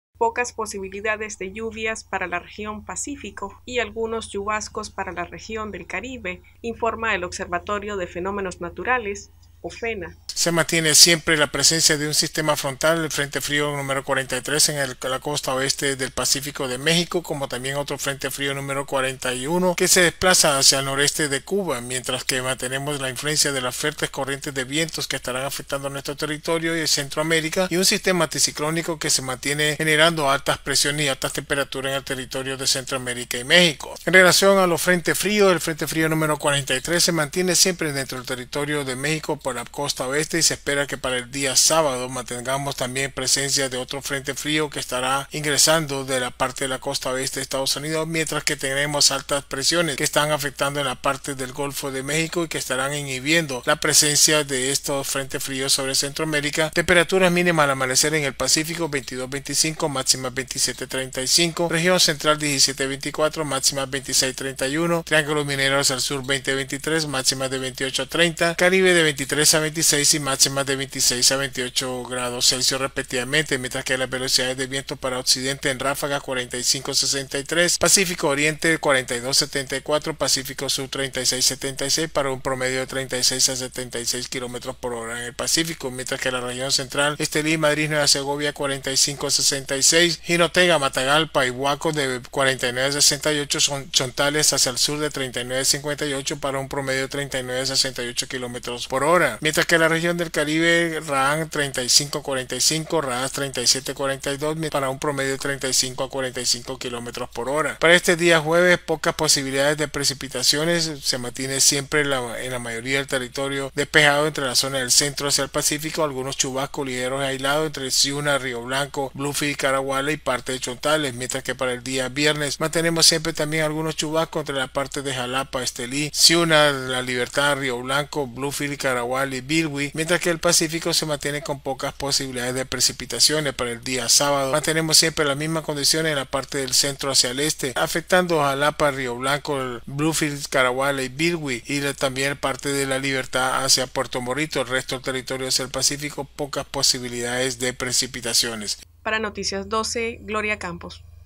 Noticias12Nicaragua Fuertes vientos afectarán el territorio nicaragüense pronostica Ofena
Fuertes vientos afectarán el territorio nicaragüense pronostica Ofena